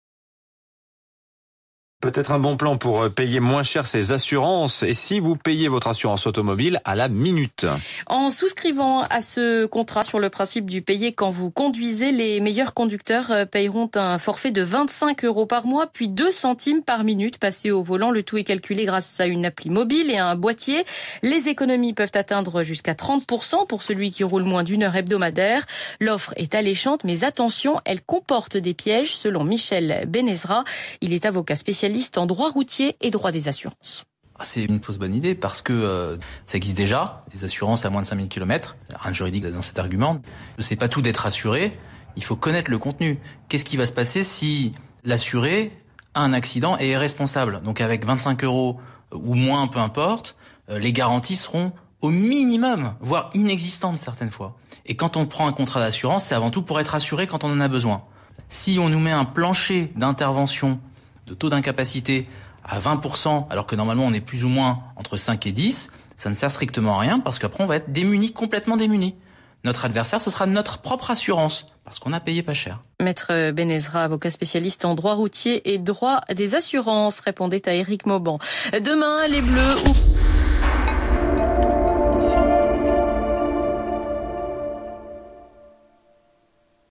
assurance a la minute, une fausse bonne idée ?
une révolution l'assurance à la minute? pas vraiment selon maître Michel benezra, avocat spécialisé en droit de l'automobile, l'affaire si cela en est une, comporte des risques importants...